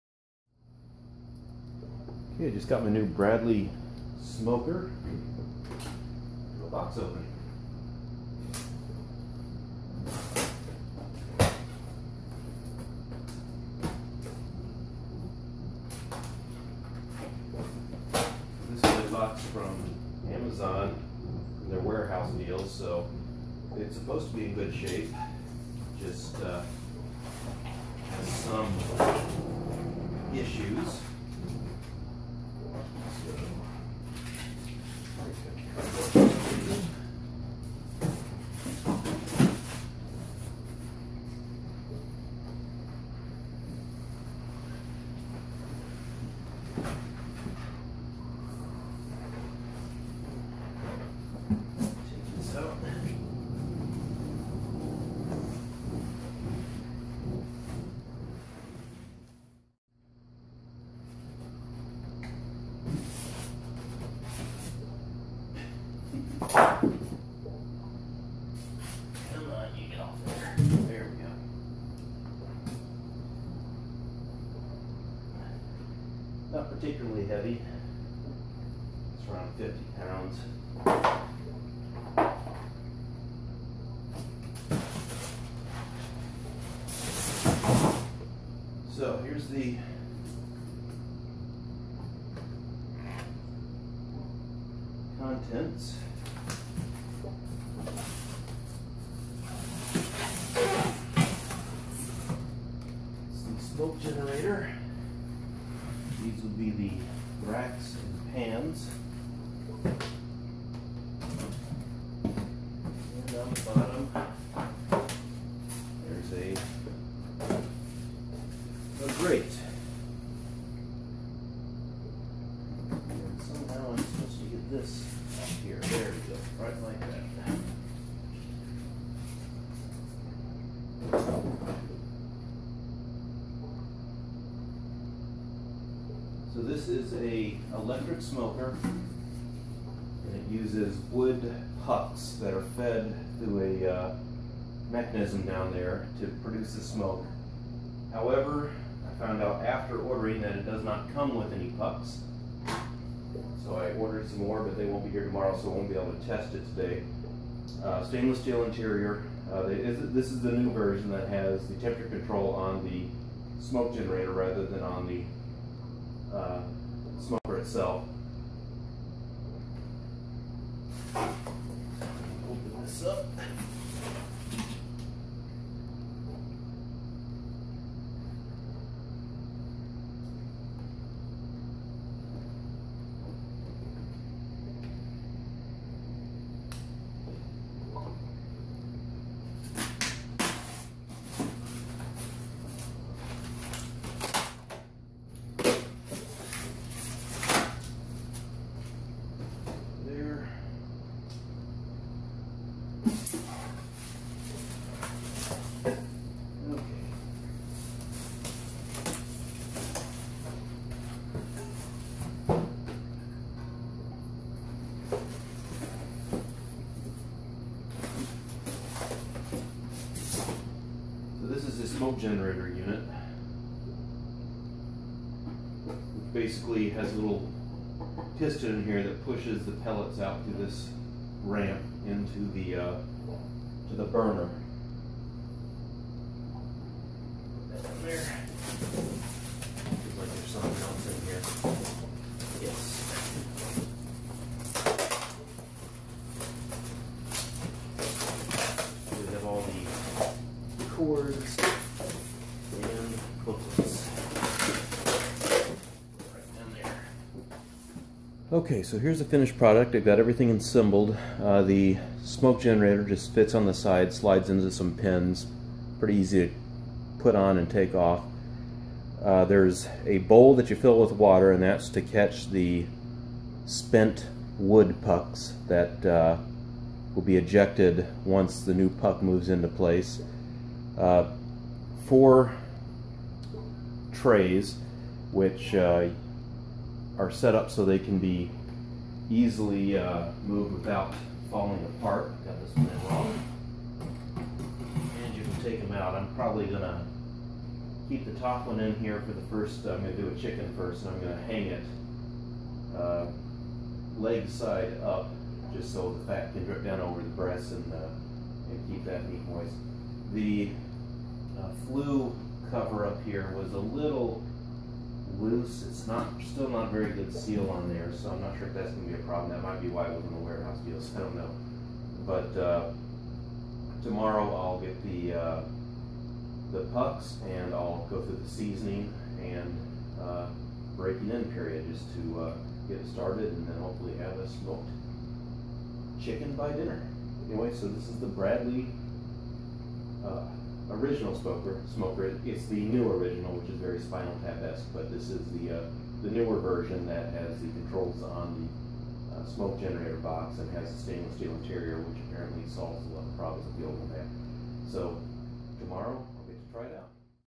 Bradley Smoker Unboxing
Opening up my new Bradly Original Smoker.